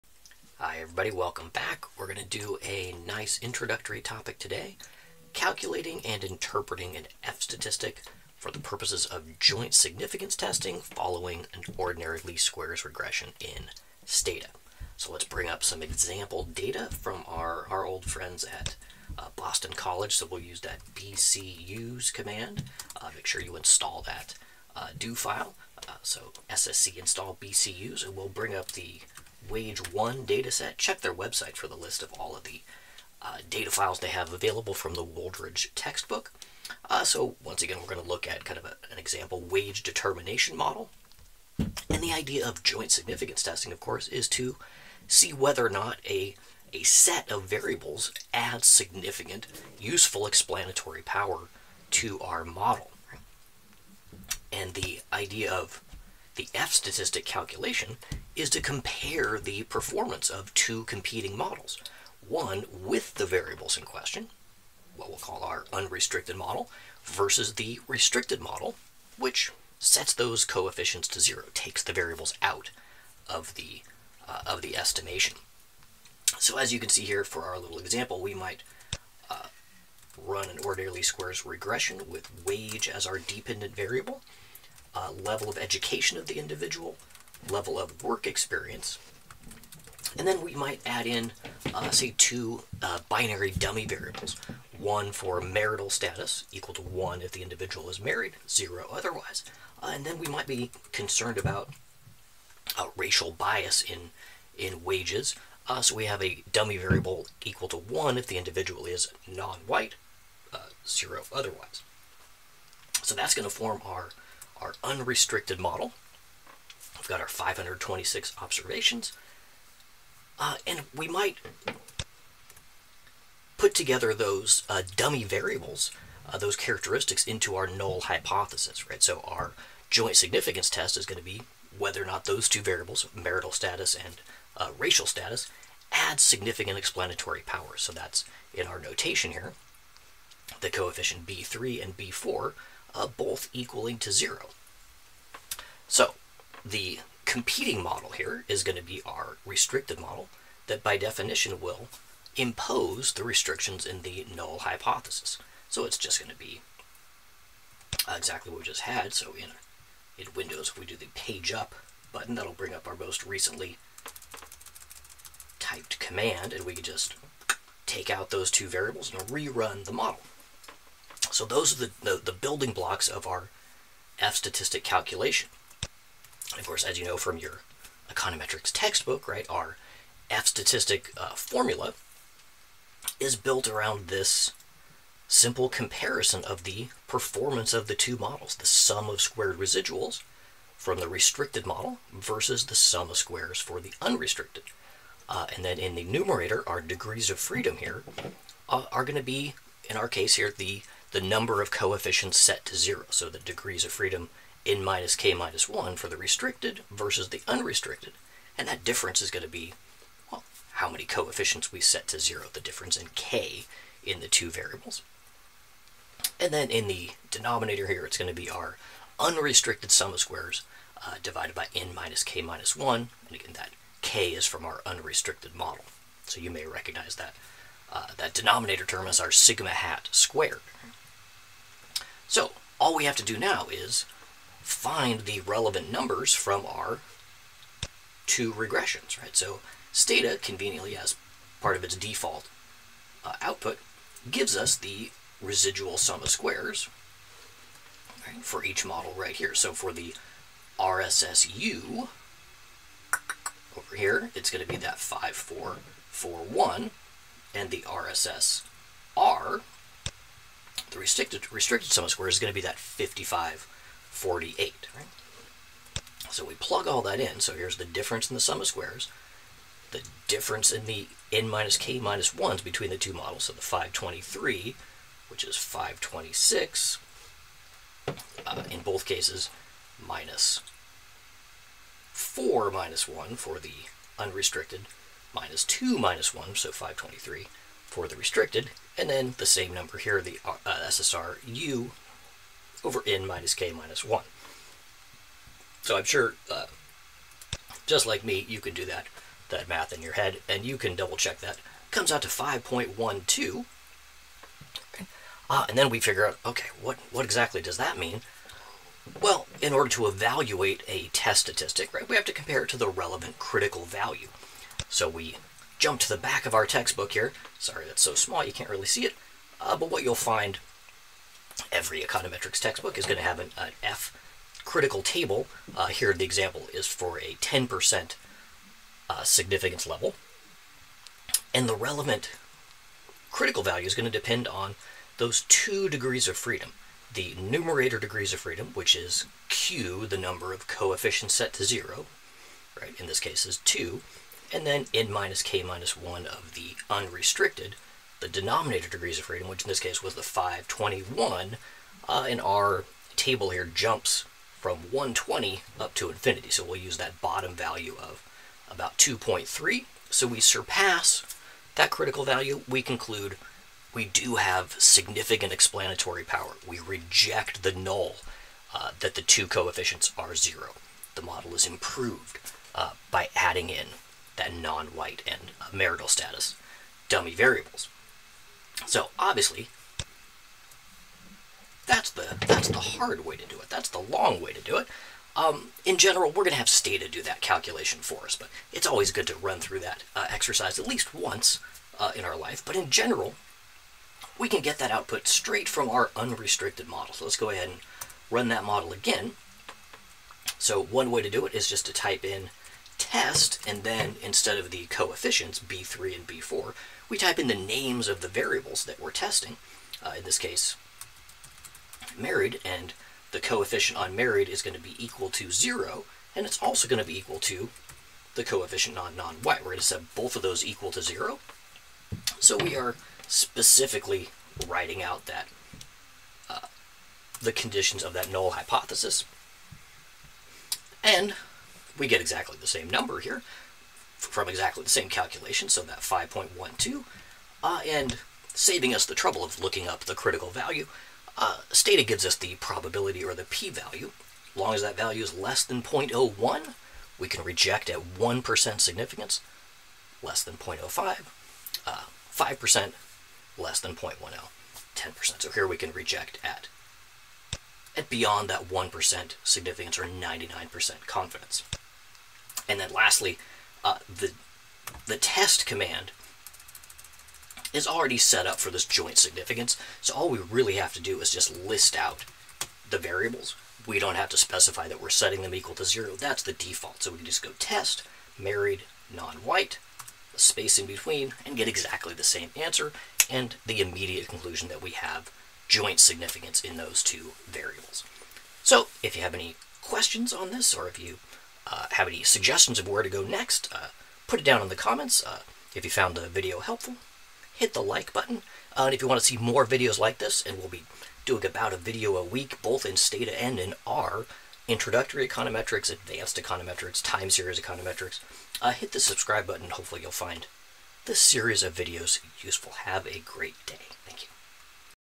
Stata Tutorial: F-statistic for joint significance testing
3 ways of calculating an F-statistic for joint significance testing in Stata, along with interpretation of results.

Link to "Gentle Introduction to Stata"

Link to the excellent Introduction to Econometrics Textbook by AH Studenmund:

Link to Jeffrey Wooldridge Introductory Econometrics Textbook: